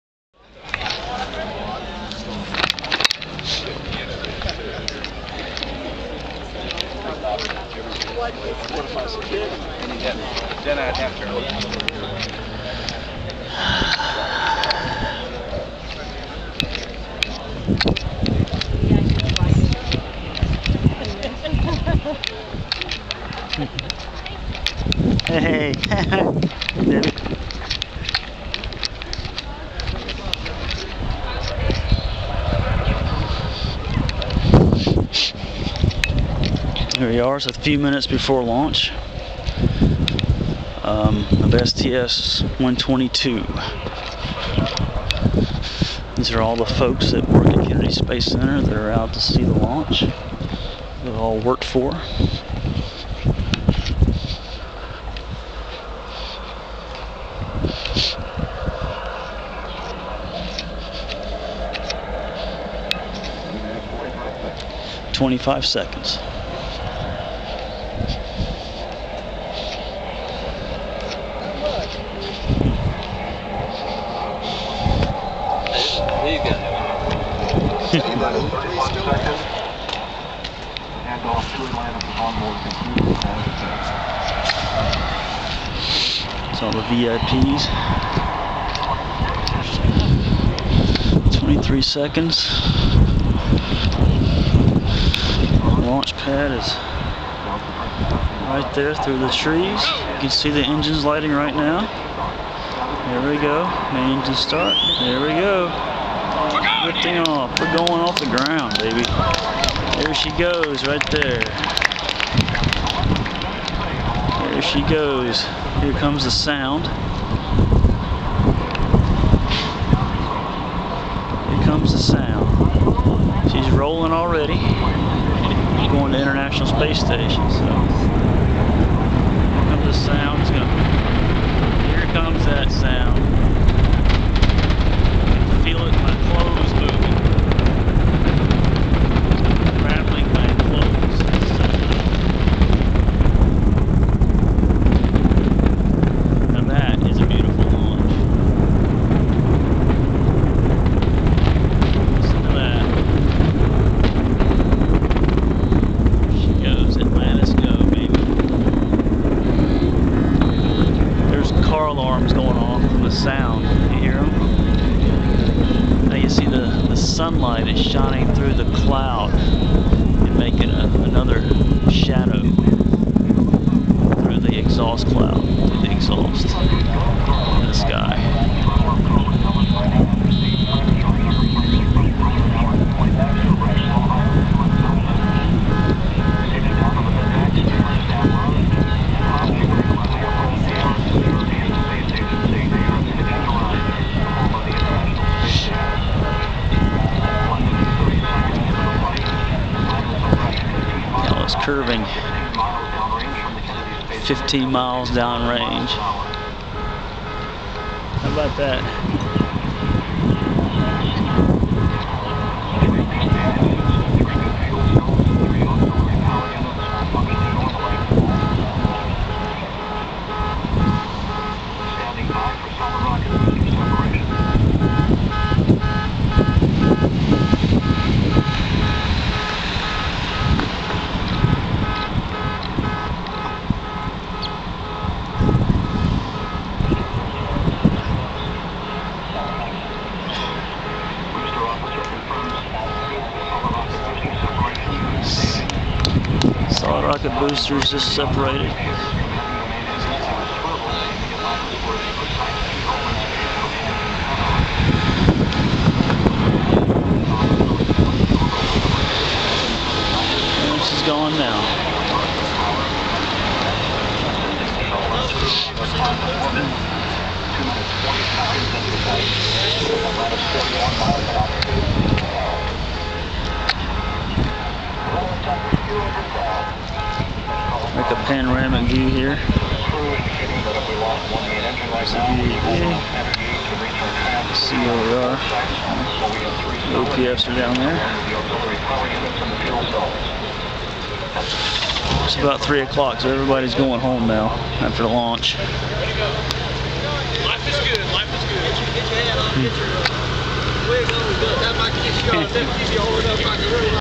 Shuttle launch STS 122 Atlantas
shuttle alantas launch video at KSC OSB 1 parking lot